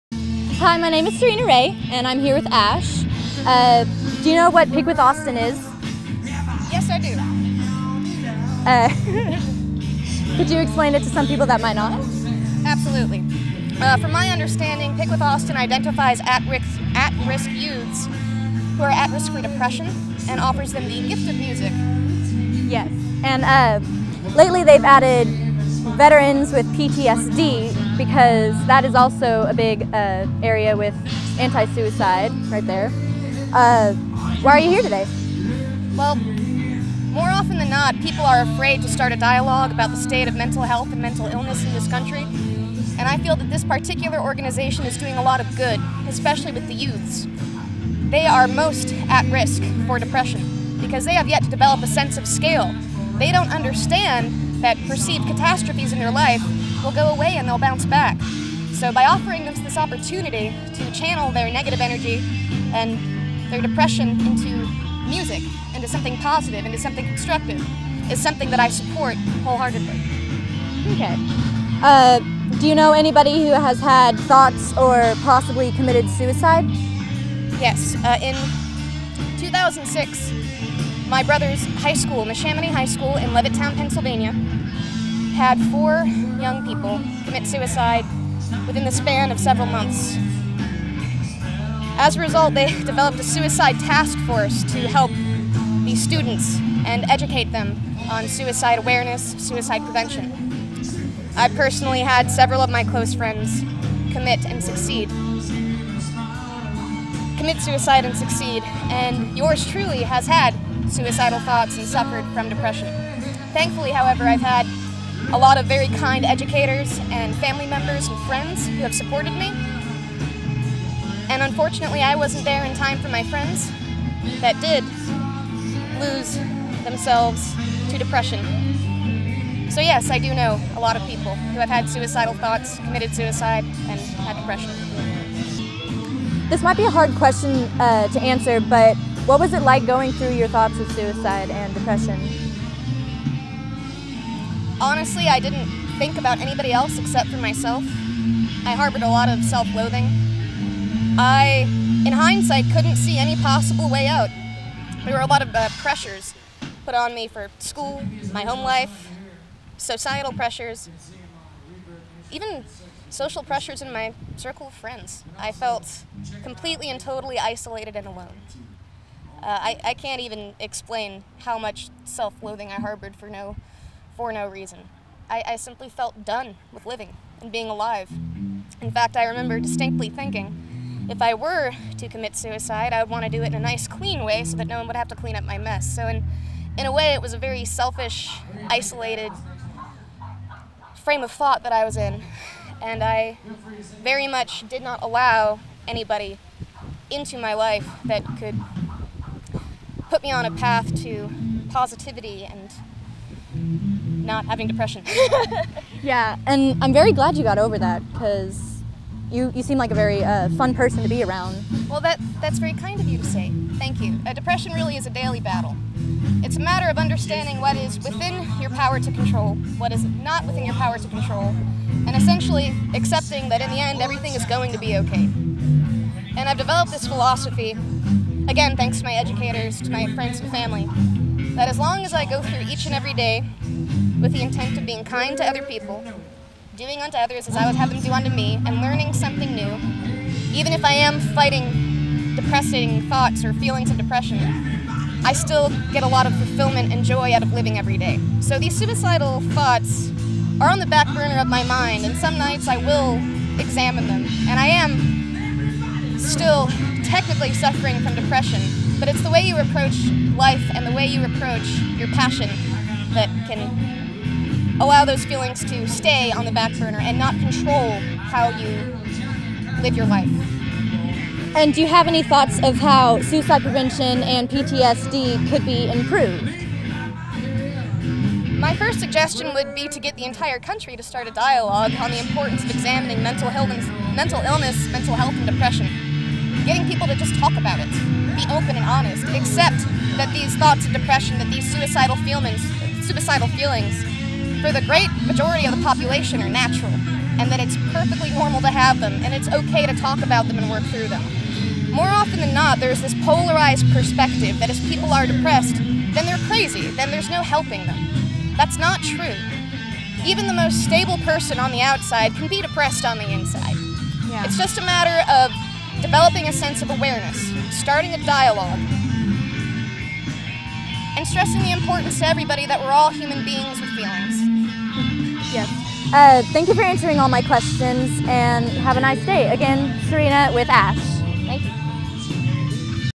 PWA's Sirena interviews Ash
May 10th, 2014 Pick With Austin awareness event at Kay's Motorcycle Mania in Nolanville, Texas.
A big thanks to Kay's for everything!

In this video Sirena interviews Ash.